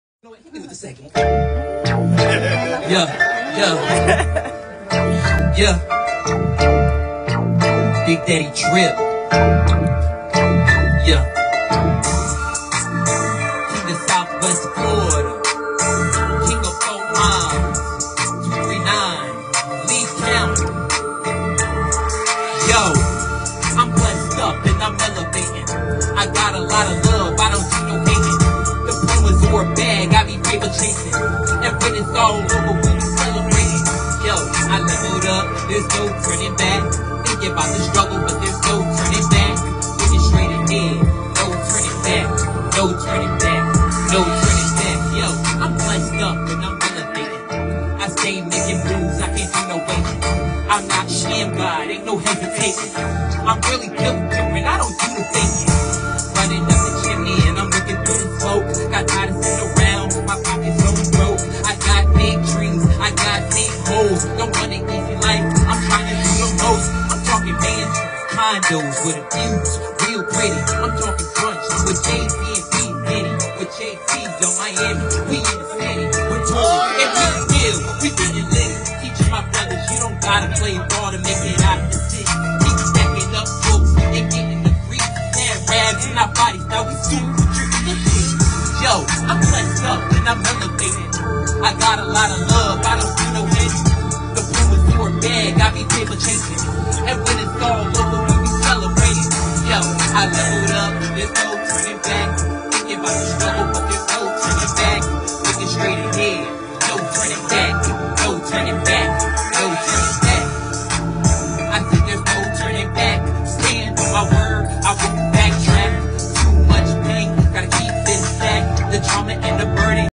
The News of The People #353/The News of The World #41: Footage of King Big Daddy Drip Performance
The News of The People Episode 353/The News of The World Episode 41: “Just Velocity Studios’ First Open Mic Night Was Phenomenal!” (The News Footage of King Big Daddy Drip’s Performance)

Here’s a word found by “The News of The People”(@theinternalsphere) and “The News of The World”(@theexternalsphere) on the first mic night hosted by Just Velocity Studios (@justvelocitystudios):

“Hear Ye! Hear Ye, ladies and gentlemen!The King 👑🤴 and The Golden Empire are doing something big this past Friday Night at Just Velocity Studios!!!

There was open mic night going down, and yours truly is going to be performing live this Friday at Just Velocity Studios! Tickets were just $20 at the door, and there was free parking!

Big shoutouts to all of the artists and the event hosts for doing an awesome job with this event from start to finish 🔥🔥🔥!!!”

- Zachariah White (@kingbigdaddydrip )
King of The Internal Sphere
King of The External Sphere
King From and Of Fort Myers
King From and Of Southwest Florida

(Video Credits - Cristina @christy2323 and “Negra” @n3grax)